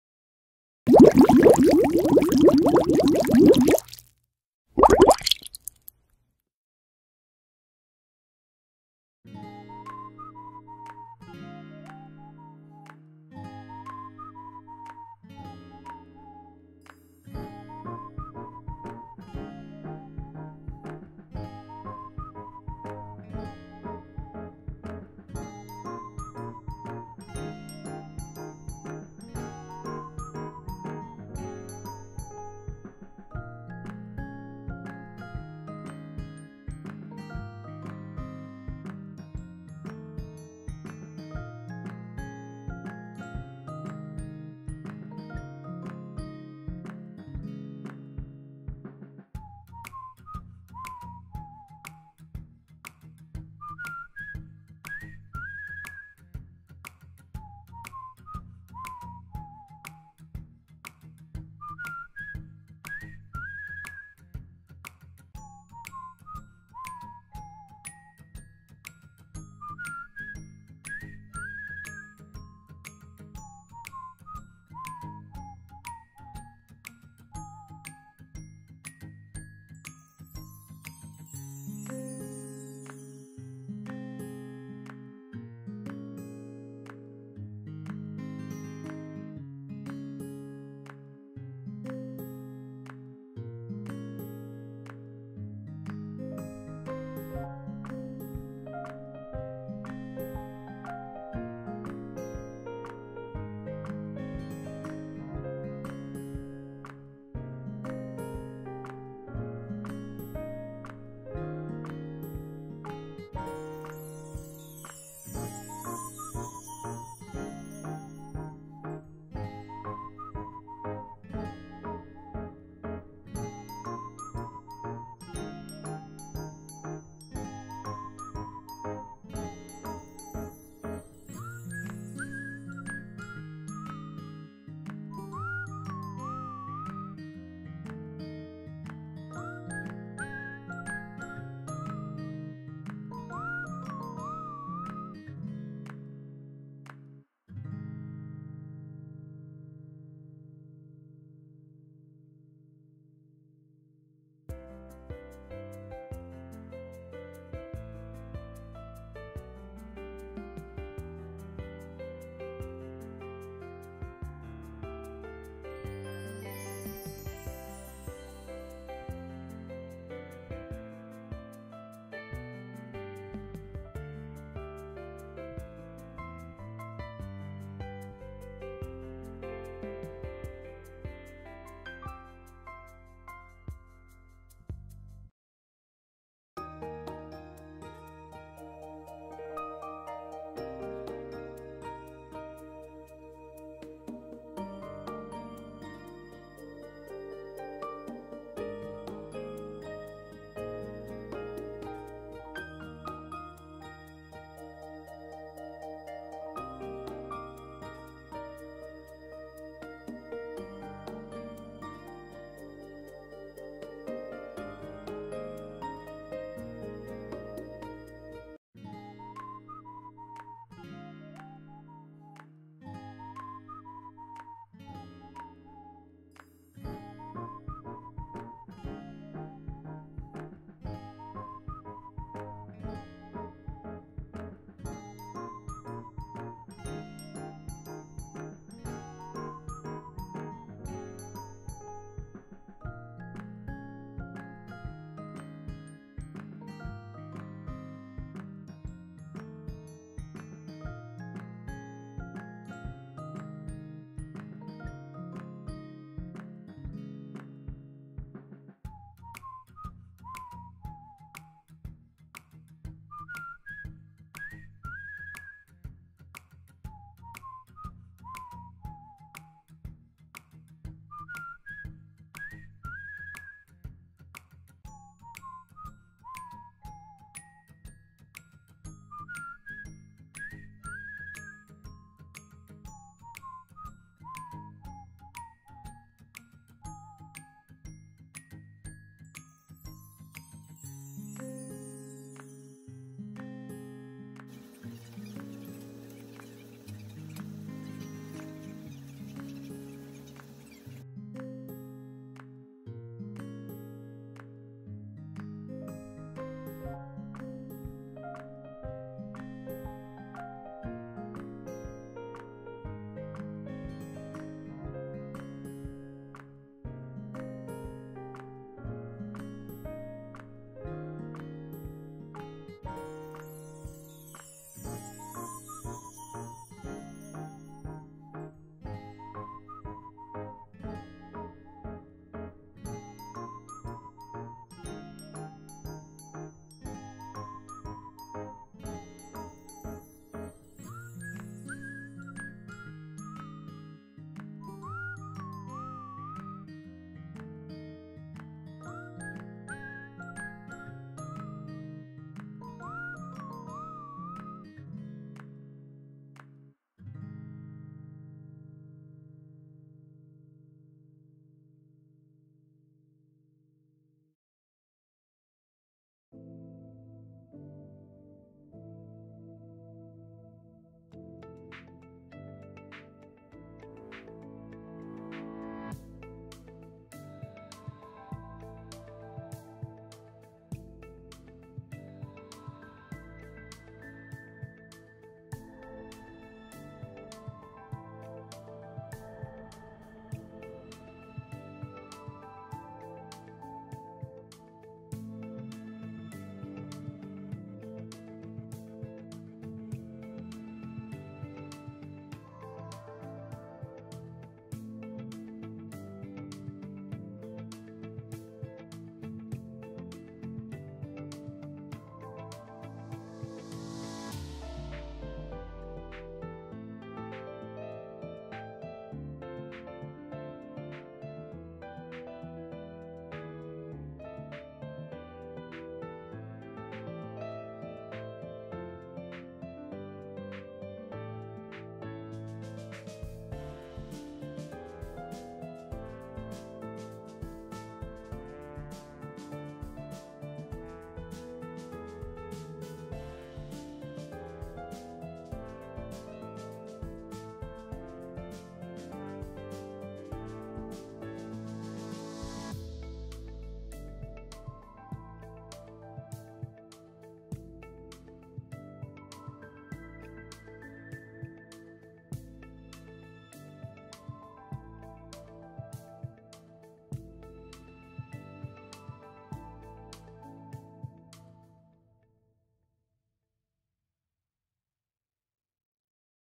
새우사료 뭐가 좋을까?
새우들이 잘 먹는 사료들 그리고 특징들을
알려드립니다. 개인적인 생각이구요. 또 사료마다
성질이 다르고 브리더 분들의 의견도 다를수 있어요.